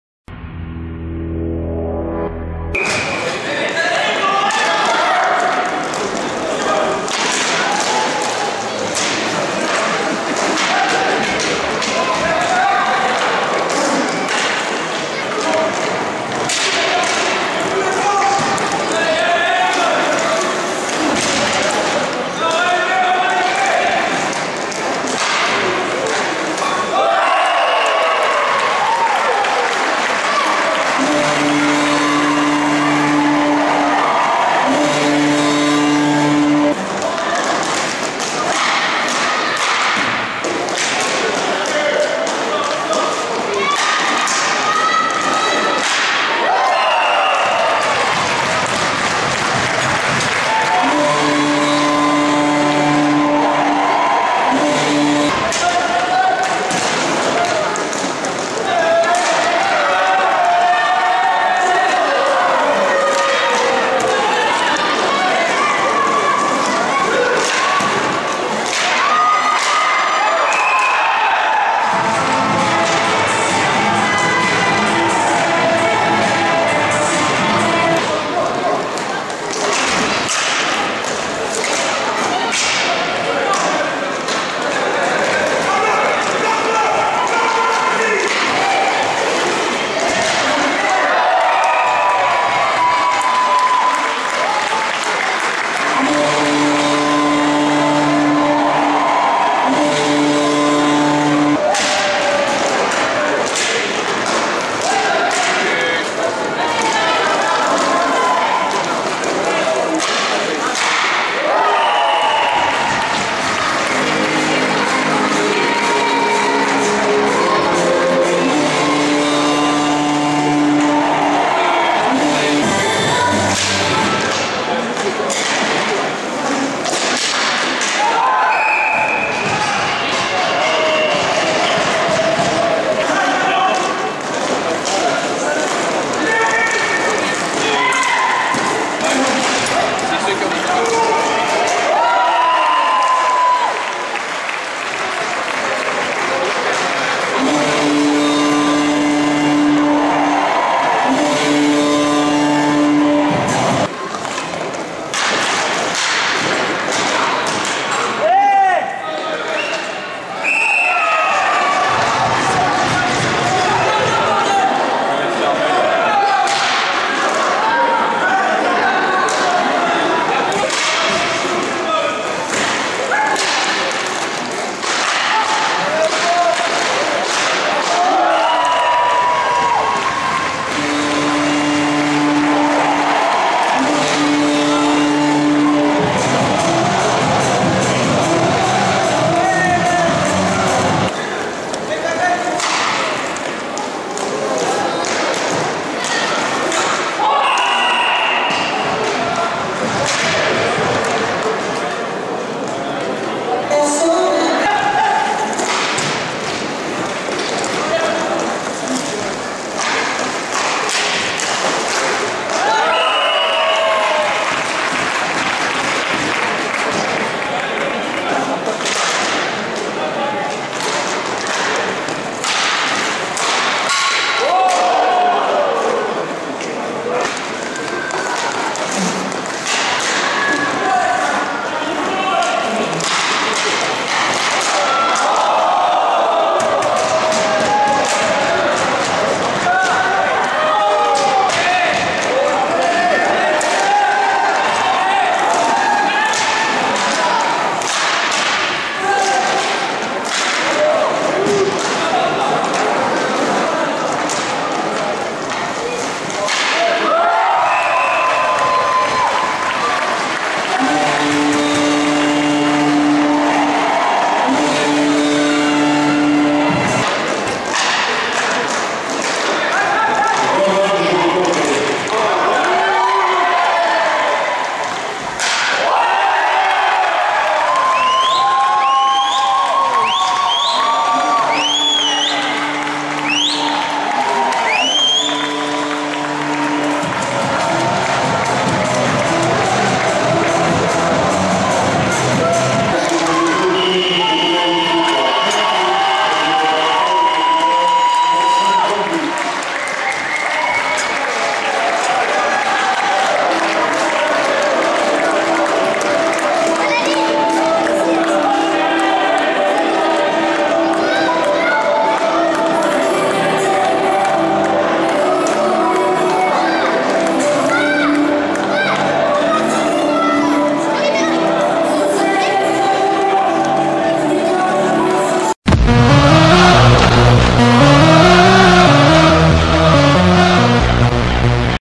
COUPE DE FRANCE 2016/2017- RESUME 1/4 FINALE YETI'S GRENOBLE FACE AUX SPIDERS DE ROUEN ROLLER HOCKEY
Victoire de Grenoble 10 à 3